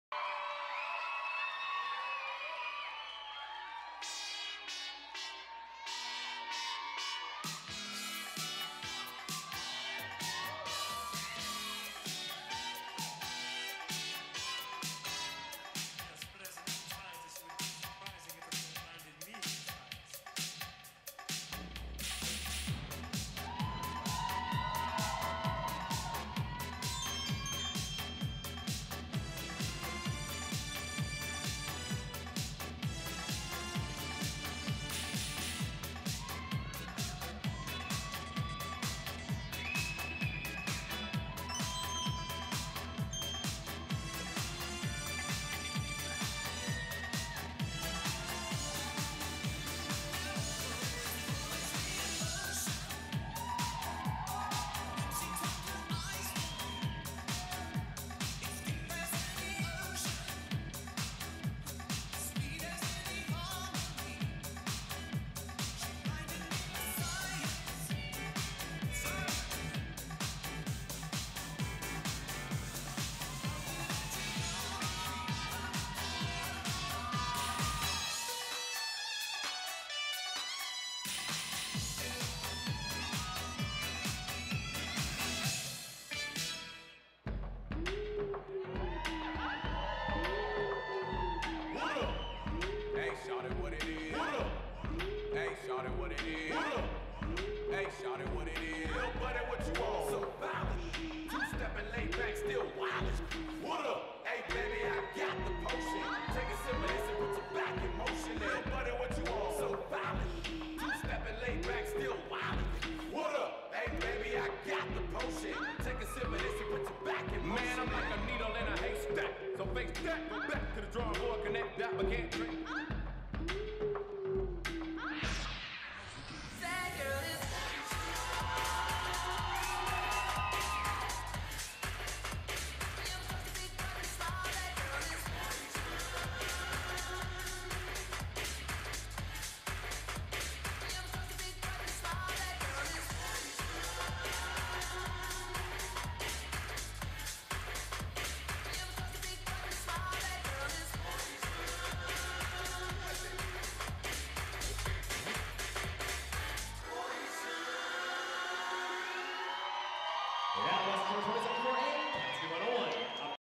"Chemistry 101"- Canadian Dance Company
CDC 2016 Intermediate Group
The Dance Awards- Mini Division
Choreographer: Samantha Lupien
Directors: Allain and Dawn Lupien
Canadian Dance Company